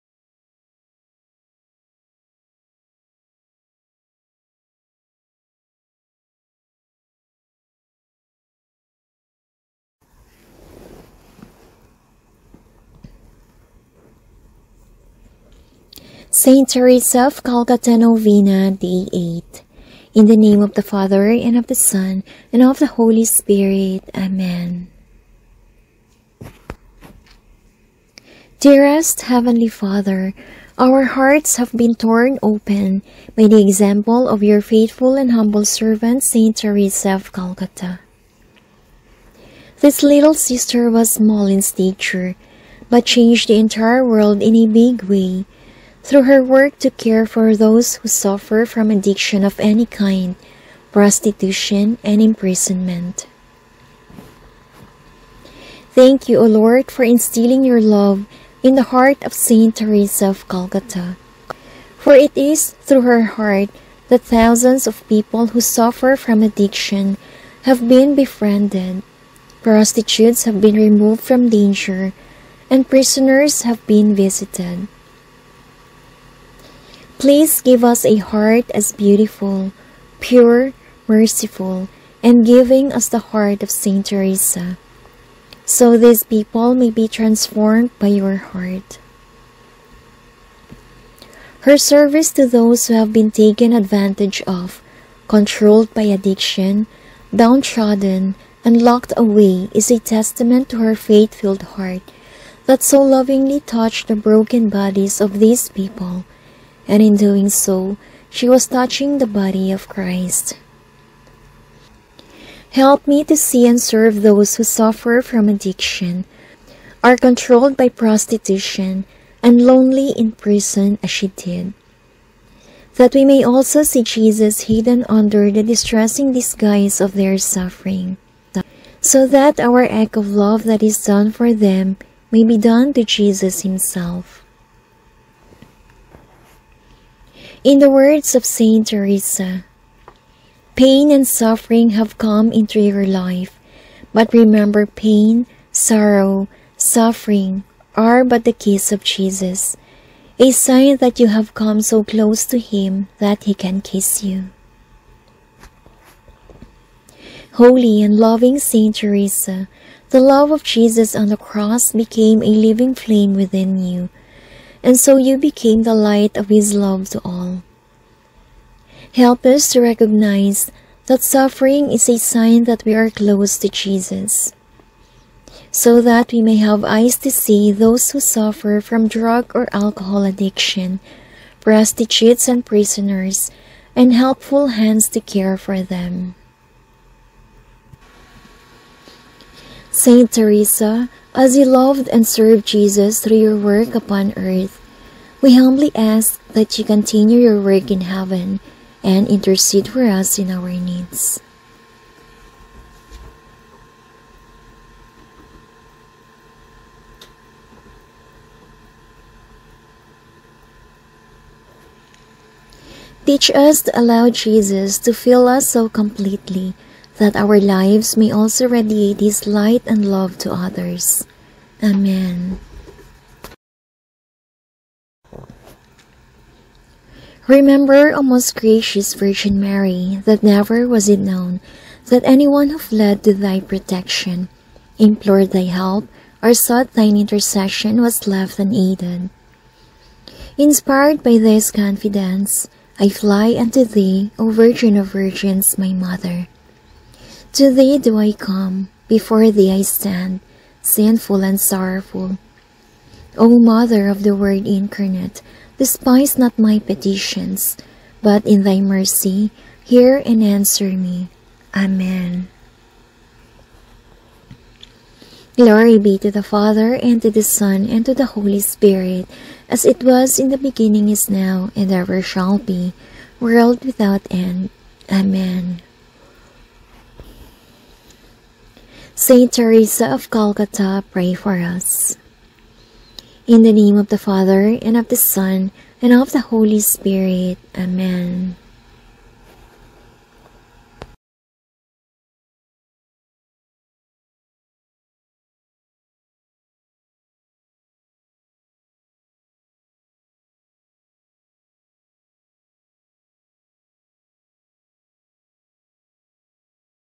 St. Teresa of Calcutta Novena : Day 8
FEAST DAY: September 5
Day 8 Novena
PATRON SAINT OF World Youth Day, Missionaries of Charity, Archdiocese of Calcutta (co-patron)

Dearest Heavenly Father,
Our hearts have been torn open by the example of your faithful and humble servant St. Teresa of Calcutta.

This little sister was small in stature but changed the entire world in a big way through her work to care for those who suffer from addiction of any kind, prostitution and imprisonment.

Thank you, O Lord, for instilling your love in the heart of St. Teresa of Calcutta, for it is through her heart that thousands of people who suffer from addiction have been befriended, prostitutes have been removed from danger and prisoners have been visited. Please give us a heart as beautiful, pure, merciful and giving as the heart of St. Teresa. So these people may be transformed by Your heart.

Her service to those who have been taken advantage of, controlled by addiction, down-trodden and locked away is a testament to her faith-filled heart that so lovingly touched the broken bodies of these people and in doing so she was touching the body of Christ.

Help me to see and serve those who suffer from addition, are controlled by prostitution and lonely in prison as she did. That we may also see Jesus hidden under the distressing disguise of their suffering. So that our act of love that is done for them may be done to Jesus himself.

In the words of St. Teresa,
“Pain and suffering have come into your life, but remember pain, sorrow, suffering are but the kiss of Jesus — a sign that you have come so close to Him that He can kiss you.”

Holy and loving St. Teresa, the love of Jesus on the Cross became a living flame within you, and so you became the light of His love to all. Help us to recognize that suffering is a sign that we are close to Jesus! So that we may have eyes to see those who suffer from drug or alcohol addiction, prostitutes and prisoners and helpful hands to care for them.

St. Teresa, as you loved and served Jesus through your work upon Earth, we humbly ask that you continue your work in heaven and intercede for us in our needs… (Mention your intentions here). Teach us to allow Jesus to fill us so completely that our lives my also radiate His light and love to others. Amen.

Remember, O most gracious Virgin Mary, that never was it known that anyone who fled to thy protection, implored thy help, or sought thine intercession was left unaided.

Inspired by this confidence, I fly unto thee, O Virgin of virgins, my mother; to thee do I come, before thee I stand, sinful and sorrowful. O Mother of the Word Incarnate, despise not my petitions, but in thy mercy hear and answer me. Amen.

Glory be to the Father and to the Son and to the Holy Spirit. As it was in the beginning is now, and ever shall be, world without end. Amen.

St. Teresa of Calcutta, pray for us!